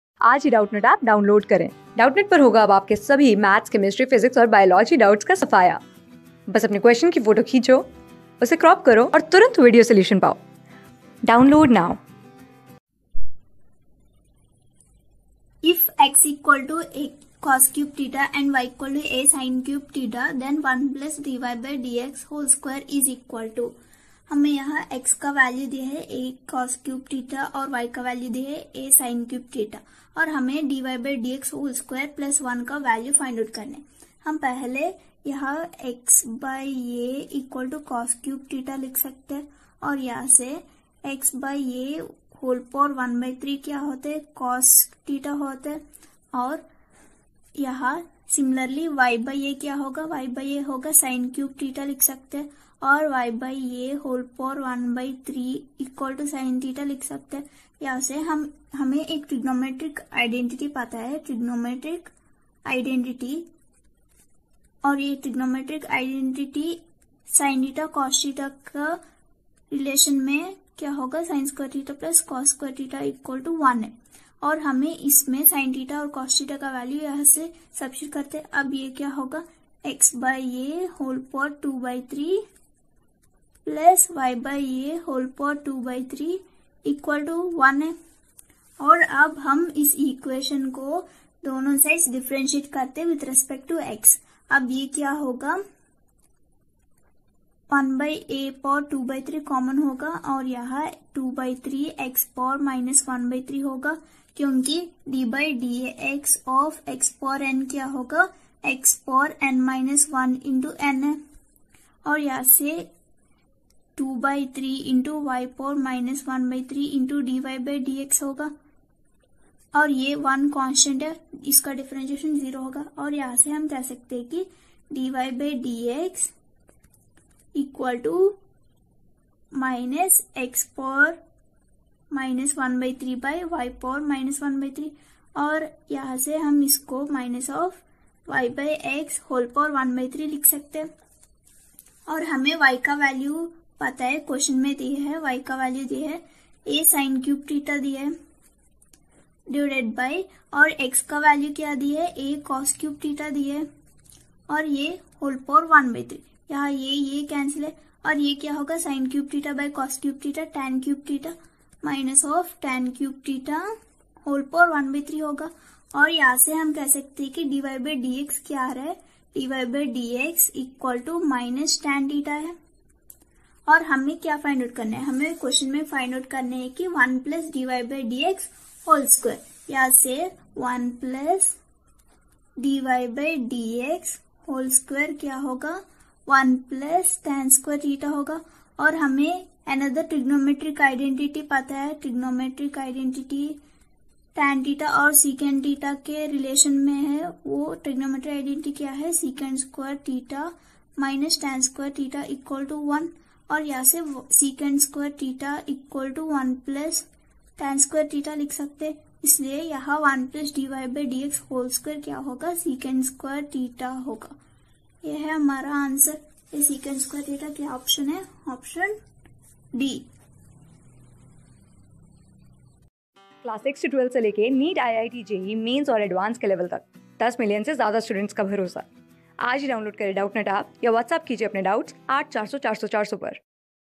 If x=a cos^3 theta and y=a sin^3 theta, then 1+(dy/dx)^2 is equal to: | CLASS 12 | SAMPLE PAPER ...
If x=a cos^3 theta and y=a sin^3 theta, then 1+(dy/dx)^2 is equal to:
Class: 12
Subject: MATHS
Chapter: SAMPLE PAPER 1
Board:CBSE

👉 Have Any Query? Ask Us.